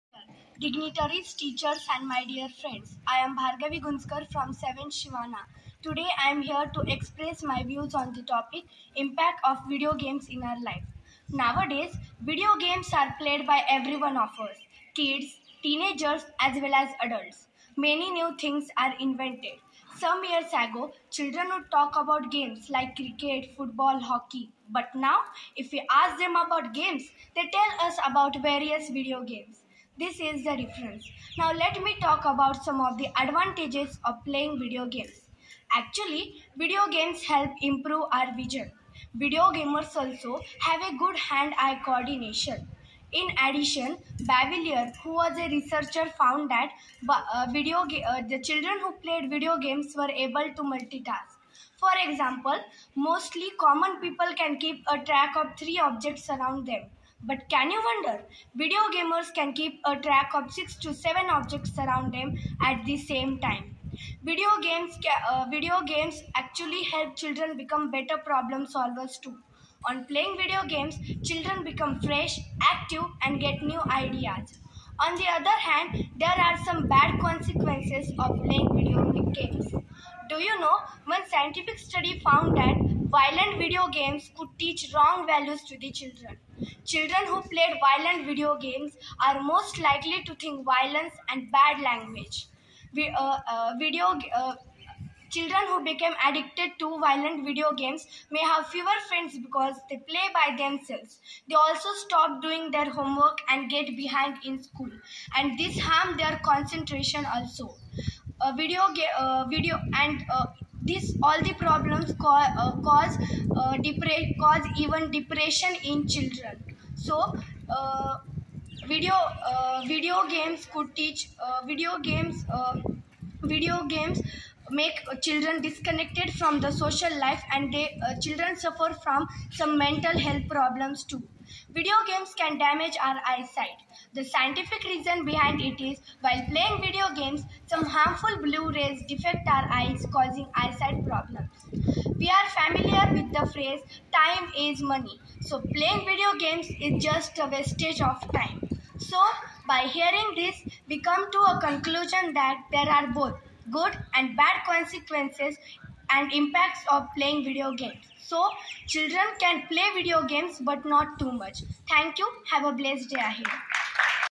Elocution Competition
Competition held on 17/08/2022. Bhargavi Gunjkar from Grade 7.